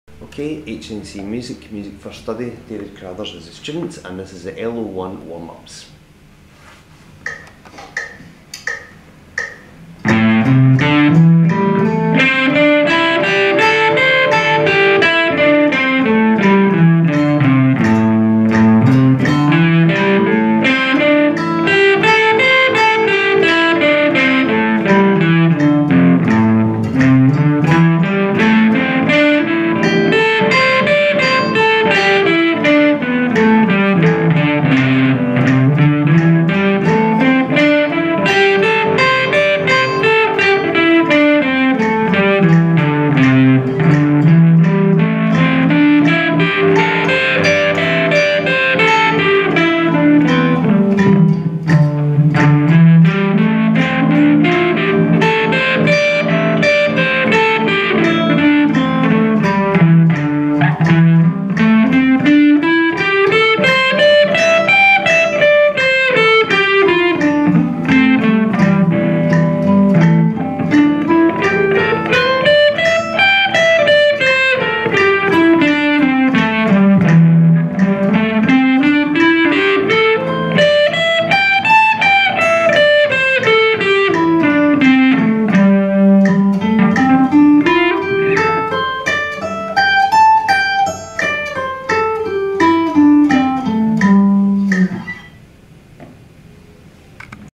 Music First Study Artefact 3 - Guitar Warm-ups
This is a play through of me going through the A Minor Pentatonic scale for my LO1 guitar warm-ups.

I am using this Music First Study artefact for my HNC Music Graded Unit.